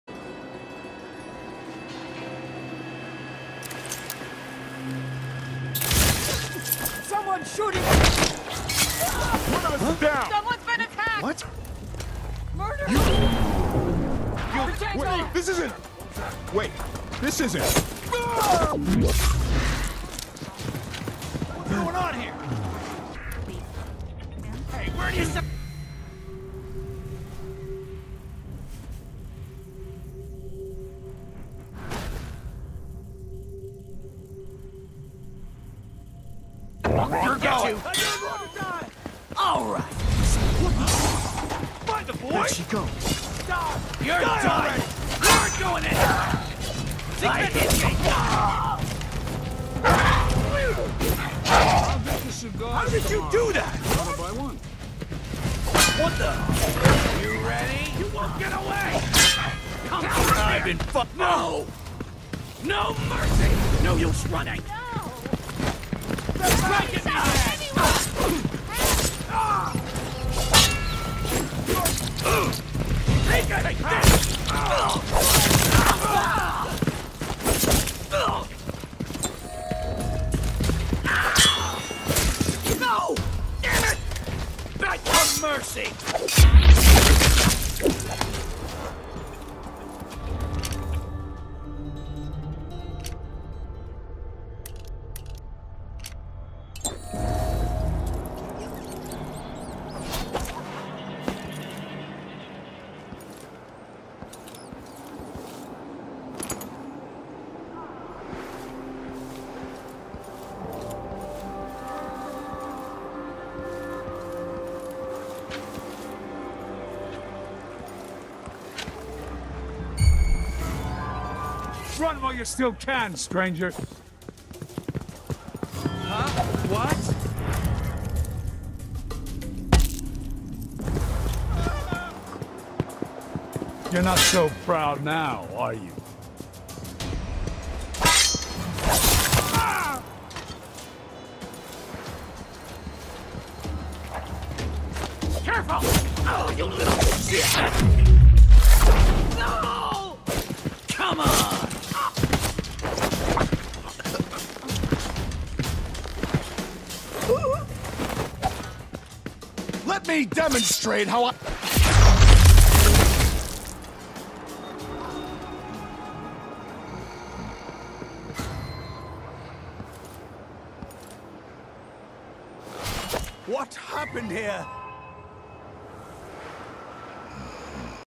Mind your business homes...
Was doing an assassination contract. Got sloppy, others who weren't involved ran up. Had to make them all go away right fast.